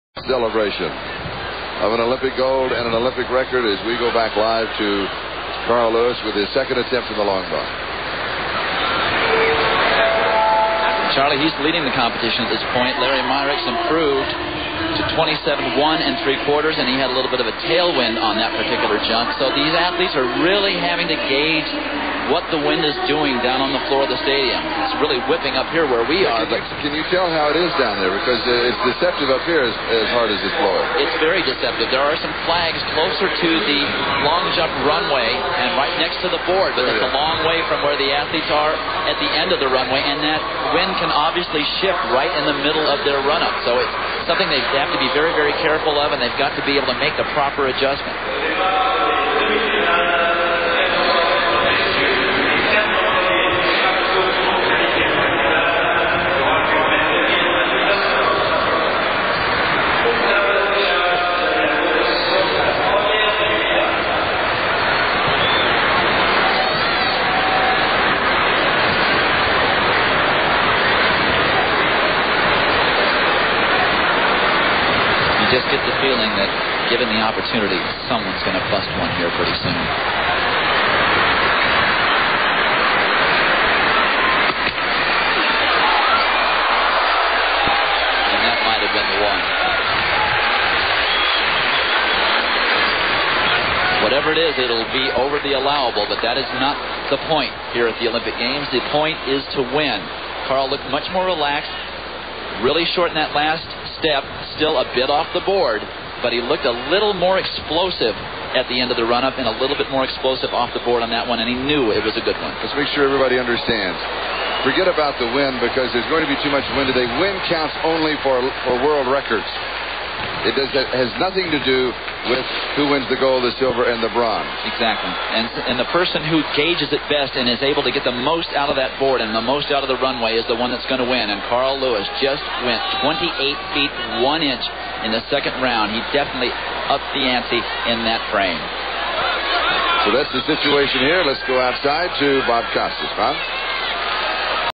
1988 Seoul Olympics Long Jump Carl Lewis
1988 Seoul Olympic Games - Men's Long Jump Final

Carl Lewis (USA)

Results - Final:

1   Carl Lewis (USA)  8.72 m
2   Mike Powell (USA)  8.49 m
3   Larry Myricks (USA)  8.27 m
4   Giovanni Evangelisti (ITA)  8.08 m
5   Antonio Corgos (ESP)  8.03 m
6   László Szalma (HUN)  8.00 m
7   Norbert Brige (FRA)  7.97 m
8   Leonid Volochine (URS)  7.89 m
9   Pang Yan (CHN)  7.86 m
10   Jarmo Kärnä (FIN)  7.82 m
11   Emiel Mellaard (NED)  7.71 m
12   Mark Forsythe (GBR)  7.54 m